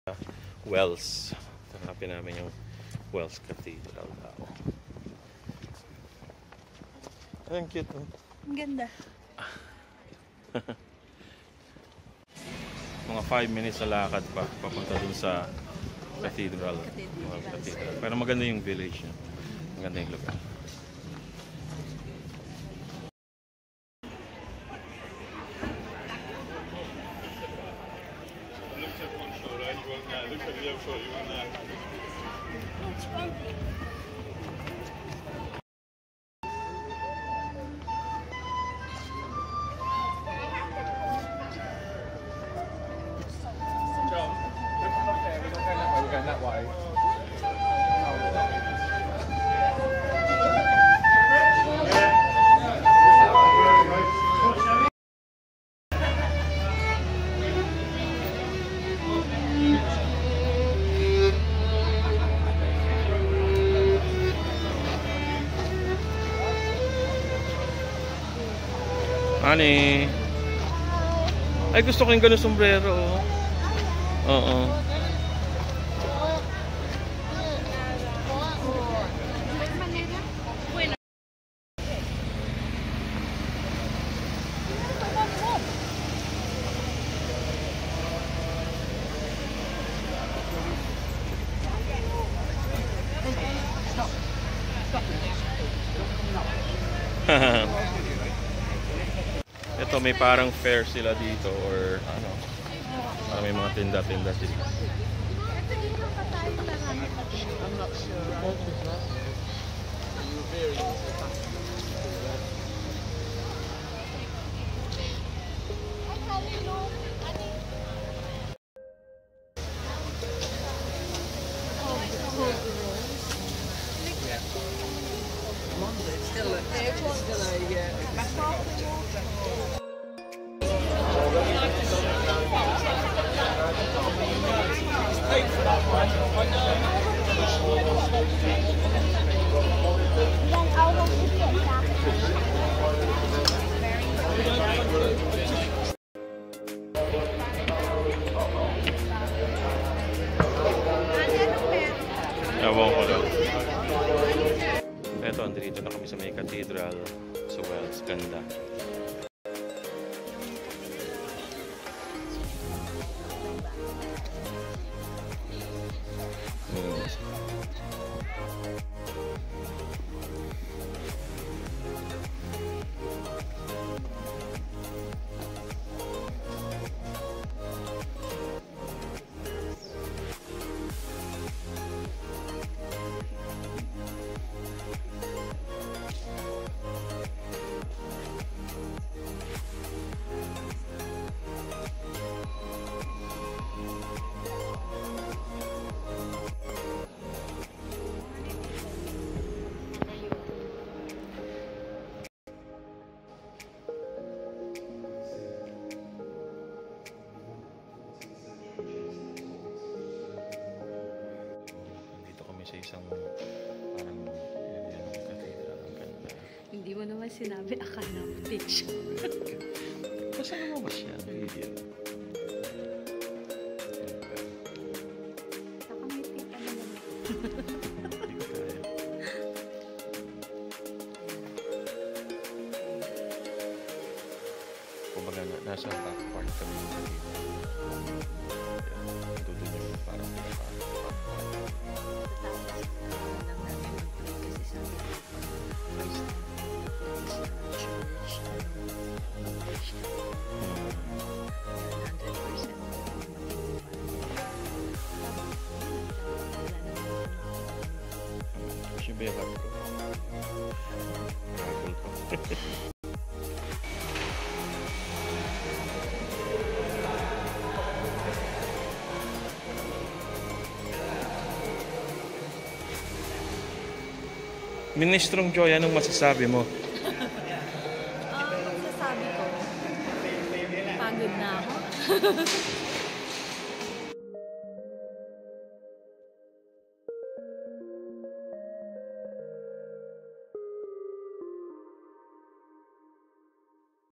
WELLS CATHEDRAL | Visiting Wells Cathedral in Somerset JayNJoy vlog 20
Wells Cathedral

We spent our second day in Somerset at Wells Cathedral. From where we parked it was a 10-minute walk. We were lucky because on the day we visited there was an event with lots of food stalls around.  Entrance is free in the Cathedral but you can give a donation. The cathedral is a  lovely and well-preserved place. We really enjoyed looking around. After this, we drove to Glastonbury and hiked up the Tor.

Please keep an eye out for our next video about the Tor.

➡️ GLASTONBURY TOR

➡️ A DAY OUT AT CHEDDAR GORGE

➡️ WARWICK CASTLE

Musician: @iksonofficial